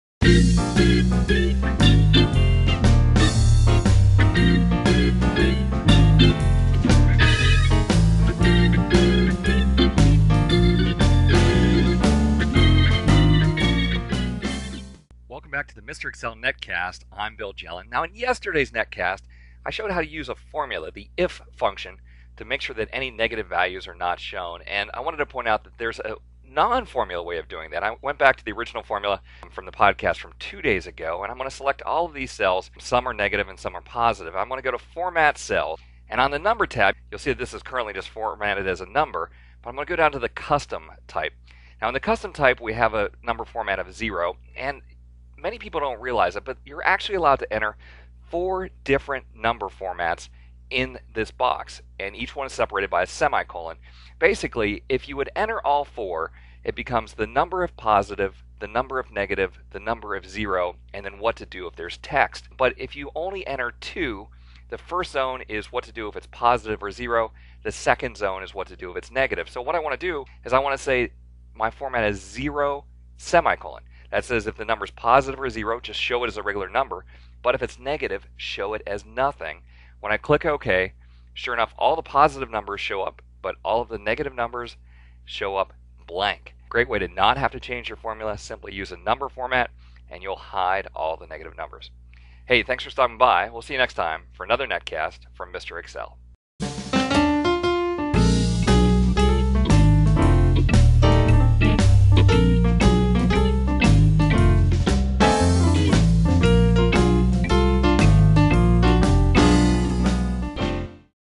Excel - Easy Way to Hide All Negative Values in Excel - Episode 553
Microsoft Excel Tutorial: Easy Way to Hide All Negative Values in Excel.
Welcome back to another MrExcel netcast! In yesterday's episode, I showed you how to use the IF function to hide any negative values in your spreadsheet. But did you know there's a non-formula way to achieve the same result? In today's video, I'll show you how to format away negatives using a simple number format.

First, let's go back to the original formula from two days ago. I've selected all the cells with both positive and negative values. Now, instead of using a formula, we'll use a number format. To do this, go to Format Cells and select the Number tab. You'll see that the cells are currently formatted as a number. We want to change this to a custom format, so scroll down and select Custom.

In the custom format box, you can enter up to four different number formats, separated by a semicolon. The first format is for positive or zero values, the second is for negative values, the third is for zeros, and the fourth is for text. In this case, we only need to enter two formats. The first one will be for positive or zero values, and the second one will be for negative values. So, we'll enter "0;" which means to show the number as is if it's positive or zero, but show nothing if it's negative. When we click OK, all the positive numbers will remain visible, but the negative numbers will disappear.

This is a great way to quickly hide all the negative numbers in your spreadsheet without having to change your formula. It's a simple number format that can save you time and effort.

(00:00) Hide Negative Values without using a formula
(00:34) Custom Number Format
(00:54) Use Semi-colon for an empty negative zone
(01:20) Clicking Like really helps the algorithm

This video answers these common search terms:
Custom type
Format cell
Formula formatting
Hide negative numbers
IF function
Negative numbers
Negative values
Non-formula way
Number format
Number tag
Positive numbers

In todays podcast, a custom number format that shows positive values and zeros, but hides the negative values. Episode 553 shows you how.